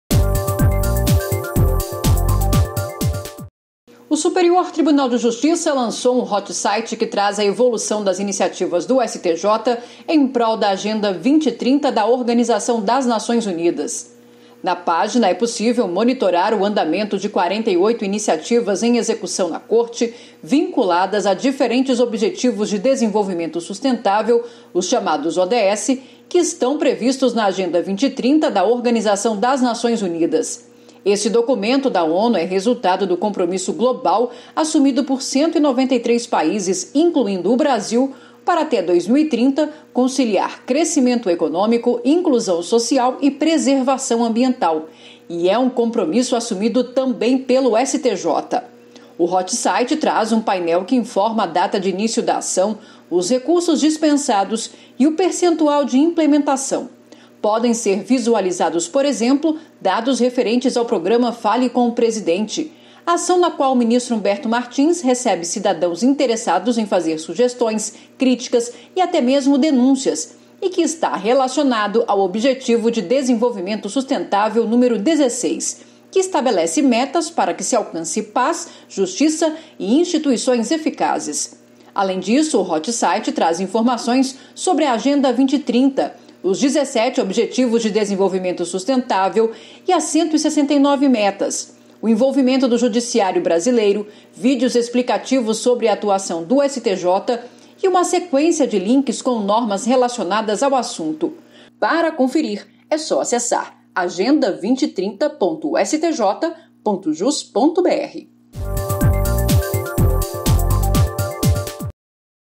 Hotsite traz evolução das iniciativas do STJ em prol da Agenda 2030 da ONU 30.06.21
​​​​​​​​O Superior Tribunal de Justiça (STJ) lançou um hotsite sobre a Agenda 2030 da Organização das Nações Unidas (ONU), por meio do qual é possível acompanhar a evolução das ações da corte vinculadas a diferentes Objetivos de Desenvolvimento Sustentável (ODS).
O documento da ONU é resultado do compromisso global assumido por 193 países – incluindo o Brasil – para, até 2030, conciliar crescimento econômico, inclusão social e preservação ambiental.